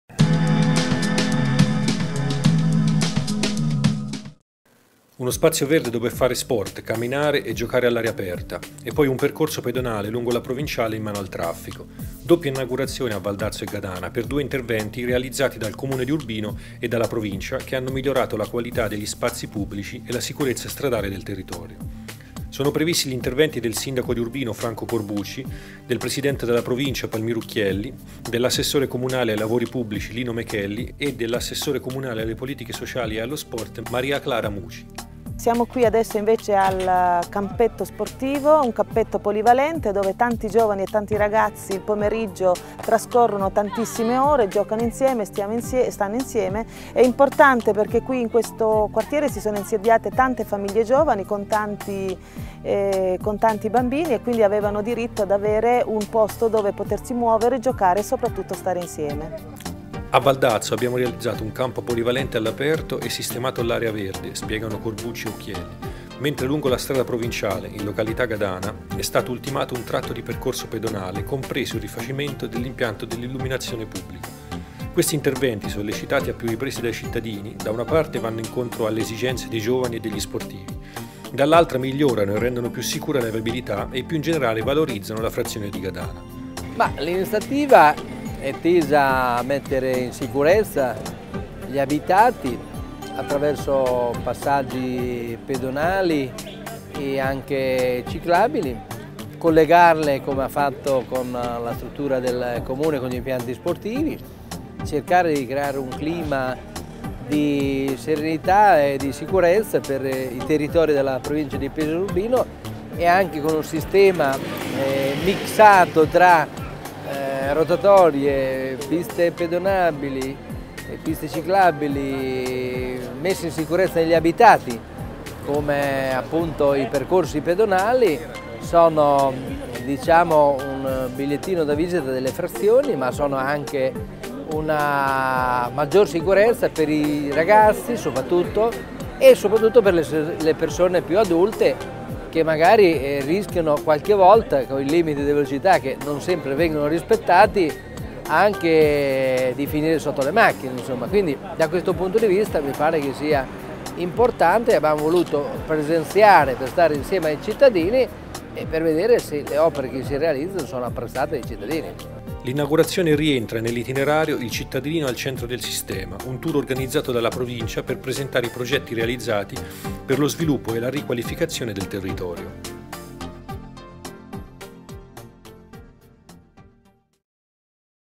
Urbino - Nuovo look per Gadana e Valdazzo
Ottobre 2008 - Uno spazio verde dove fare sport, camminare e giocare all'aria aperta. E poi un percorso pedonale lungo la provinciale in mano al traffico. Doppia inaugurazione a Valdazzo e Gadana per due interventi realizzati dal Comune di Urbino e dalla Provincia in tandem che hanno migliorato la qualità degli spazi pubblici e la sicurezza stradale del territorio.

Con il taglio del nastro dell'Area verde attrezzata e del Campo polivalente a Valdazzo. Poi la cerimonia si sposta a Gadana dove è attesa l'inaugurazione del Percorso pedonale della Feltresca.

Sono intervanuti il sindaco di Urbino Franco Corbucci, il presidente della Provincia Palmiro Ucchielli, l'assessore provinciale ai lavori pubblici Giuseppe Lucarini, l'assessore comunale ai lavori pubblici Lino Mechelli e l'assessore comunale alle politiche sociali e allo sport Maria Clara Muci.

"A Valdazzo abbiamo realizzato un campo polivalente all'aperto e sistemato l'area verde - spiegano Corbucci e Ucchielli - mentre lungo la strada provinciale, in località Gadana, è stato ultimato un tratto di percorso pedonale compreso il rifacimento dell'impianto d'illuminazione pubblica. Questi interventi, sollecitati a più riprese dai cittadini, da una parte vanno incontro alle esigenze dei giovani e degli sportivi, dall'altra migliorano e rendono più sicura la viabilità, e più in generale valorizzano la frazione di Gadana".

L'inaugurazione rientra nell'itinerario "Il cittadino al centro del sistema", un tour organizzato dalla Provincia per presentare i progetti realizzati per lo sviluppo e la riqualificazione del territorio.